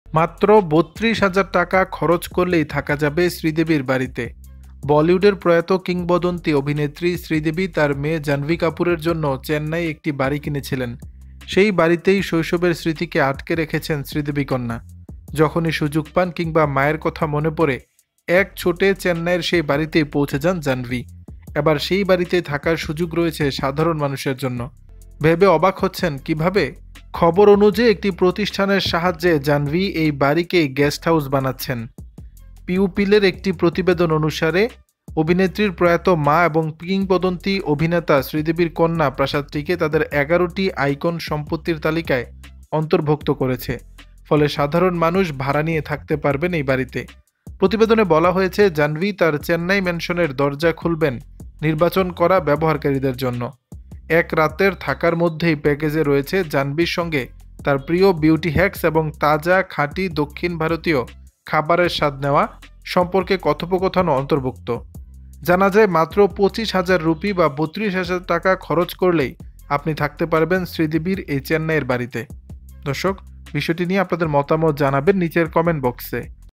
৩২ হাজার টাকায় থাকা যাবে শ্রীদেবীর বাড়িতে। জাহ্নবীর সাথে ডিনার করার সুযোগ পাবে কারা দেখুন
৩২ হাজার টাকায় থাকা যাবে শ্রীদেবীর বাড়িতে। জাহ্নবীর সাথে ডিনার করার সুযোগ পাবে কারা দেখুন

ˍˍˍˍˍˍˍˍˍˍˍˍˍˍˍˍˍˍˍˍˍˍˍˍˍˍˍˍˍˍˍˍˍˍˍˍˍˍˍˍˍˍˍˍˍˍˍˍˍˍˍˍˍˍˍˍ

ˍˍˍˍˍˍˍˍˍˍˍˍˍˍˍˍˍˍˍˍˍˍˍˍˍˍˍˍˍˍˍˍˍˍˍˍˍˍˍˍˍˍˍˍˍˍˍˍˍˍˍˍˍˍˍˍ

ˍˍˍˍˍˍˍˍˍˍˍˍˍˍˍˍˍˍˍˍˍˍˍˍˍˍˍˍˍˍˍˍˍˍˍˍˍˍˍˍˍˍˍˍˍˍˍˍˍˍˍˍˍˍˍˍˍ

ˍˍˍˍˍˍˍˍˍˍˍˍˍˍˍˍˍˍˍˍˍˍˍˍˍˍˍˍˍˍˍˍˍˍˍˍˍˍˍˍˍˍˍˍˍˍˍˍˍˍˍˍˍˍˍˍˍ

ˍˍˍˍˍˍˍˍˍˍˍˍˍˍˍˍˍˍˍˍˍˍˍˍˍˍˍˍˍˍˍˍˍˍˍˍˍˍˍˍˍˍˍˍˍˍˍˍˍˍˍˍˍˍˍ